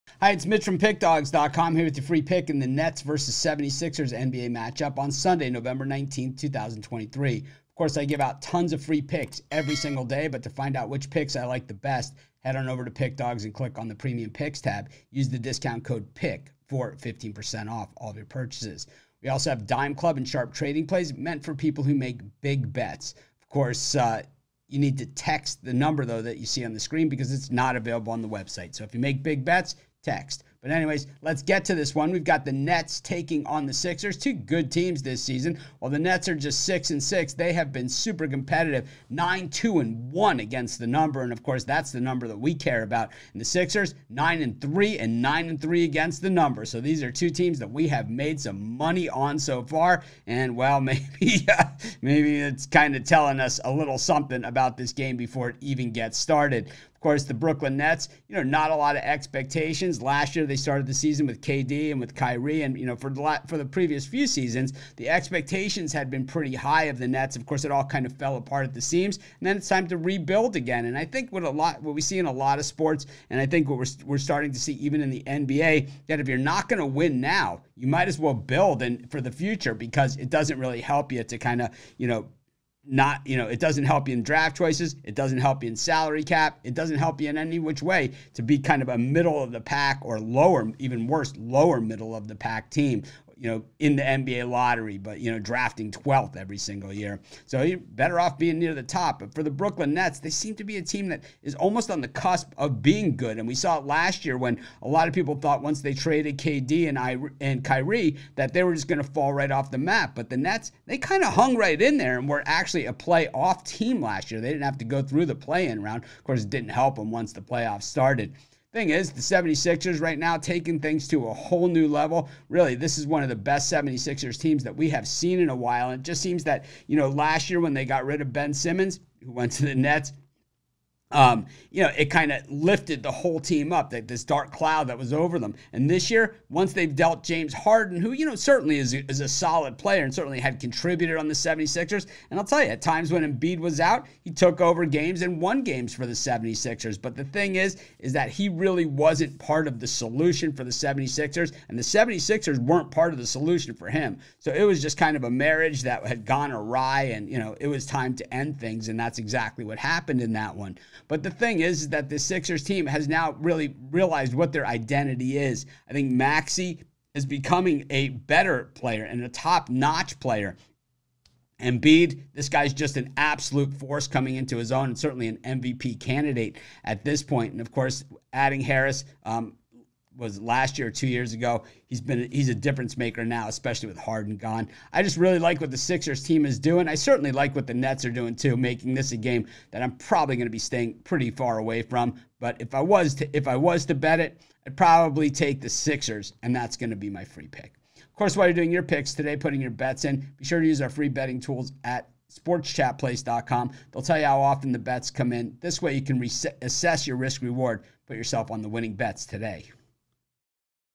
Brooklyn Nets vs Philadelphia 76ers 11/19/23 NBA Free Picks & Predictions | NBA Betting Tips
The Philadelphia 76ers face the Brooklyn Nets in Sunday's NBA Basketball action. Get Mitch's NBA Pick and Prediction for this game.